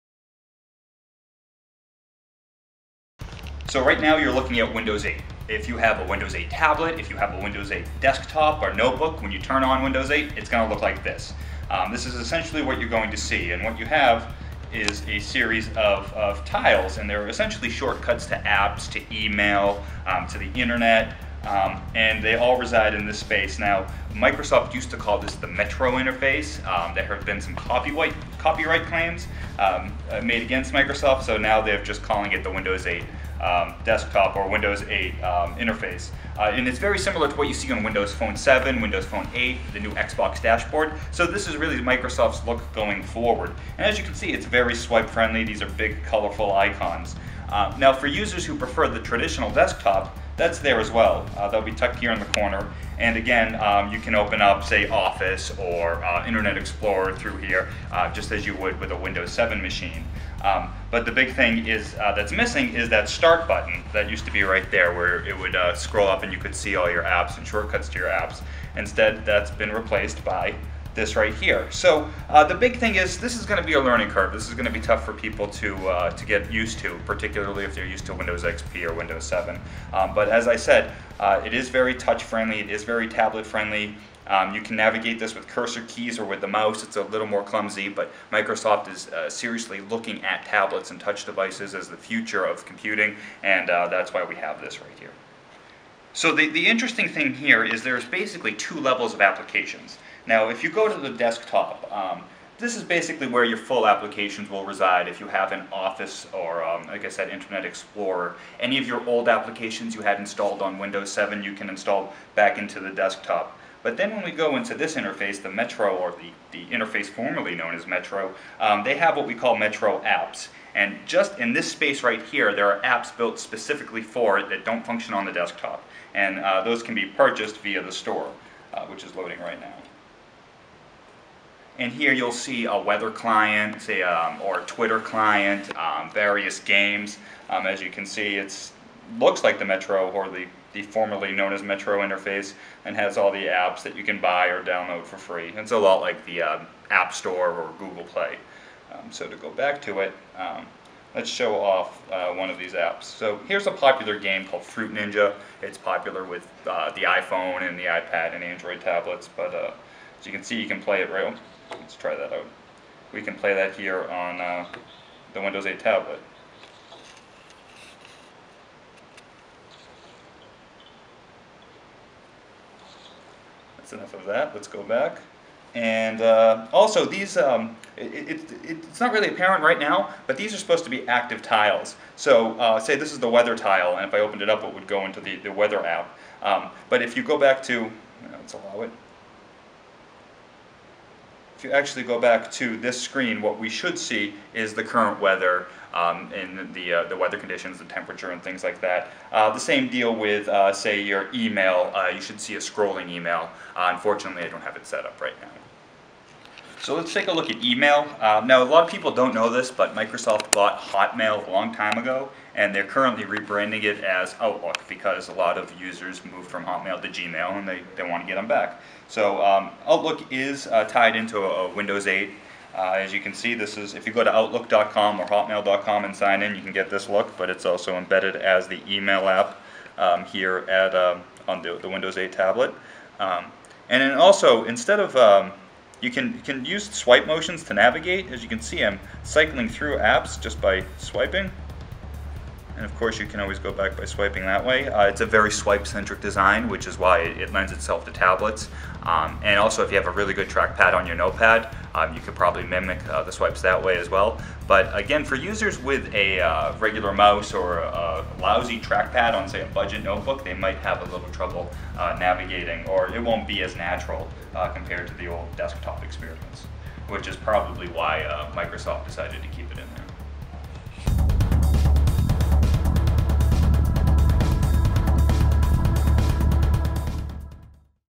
Windows 8 Video Review: Top Windows 8 Features and Functions - TabletPCReview
After more than a year of speculation, sneak peeks and early development releases, Microsoft's Windows 8 operating system will be announced and available this week.  The new OS is completely different from the company's previous products especially in terms of the user interface, touch integration and interactivity among the different elements of the software.  It also more intensely integrates a user's social activities, allowing for more collaboration and a wider flow of Web-based information across a user's desktop and within the applications themselves.

TechnologyGuide Chief Editor Jamison Cush has been following Windows 8 development and takes the OS on a  test spin on a Samsung tablet.  In the following video, he provides a look at some of Windows 8 highlights and key features.